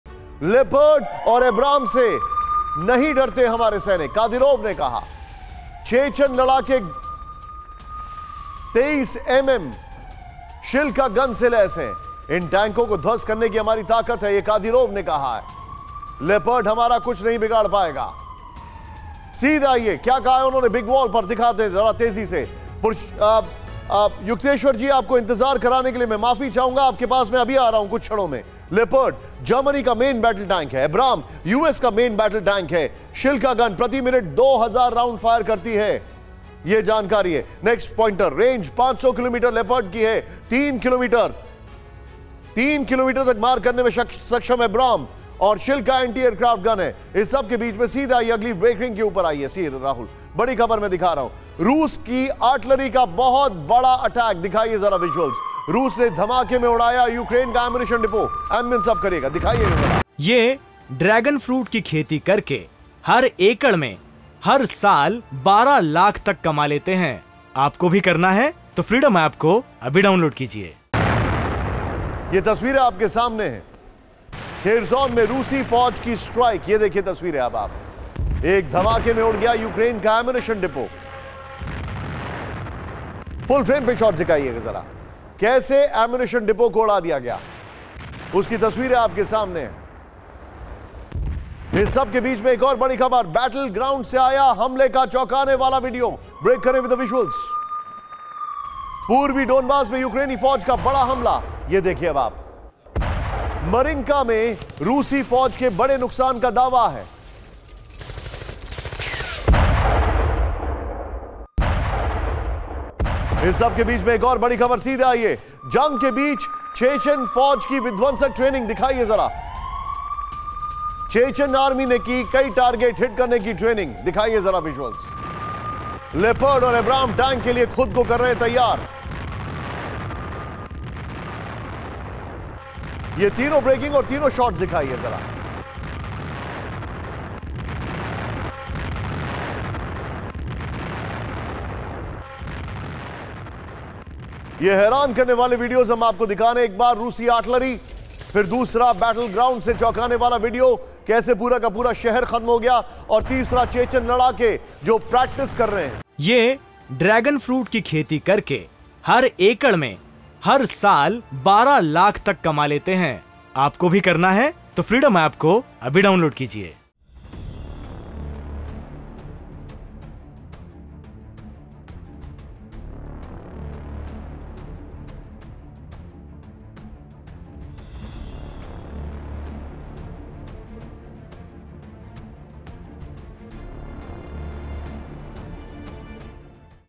Russia Ukraine war: Leopard and Abrams से नहीं डरते हमारे सैनिक- Kadyrov
चेचन कमांडर ने पश्चिम को दी खुली चेतावनी. लेपर्ड और अबराम्स से नहीं डरते हमारे सैनिक- Kadyrov. 'चेचन लड़ाके 23mm शिल्का गन से लैस हैं'. इन टैंकों को ध्वस्त करने की हमारी ताकत-कादिरोव. 'लेपर्ड-अबराम्स हमारा कुछ नहीं बिगाड़ सकते'